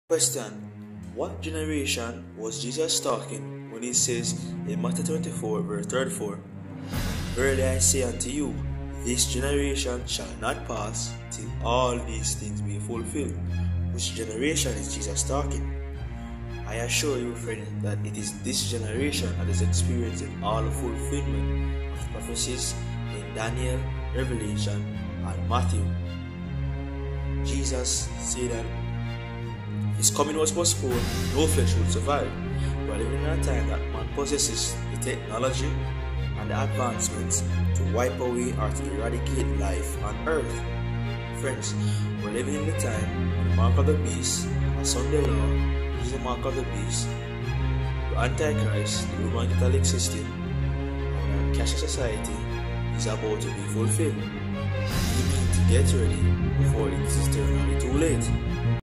Questions: Which Generation Is The final Generation Before Jesus Returns?
Which Generation is the final generation?
Matthew  24:34 Verily I say unto you, This generation shall not pass, till all these things be fulfilled.

What do you think about Matthew 24:34?

What is the Mark of the beast by- Nickyle Wilson
Reveling what's Under the Sheet Series by- Brother Teavauon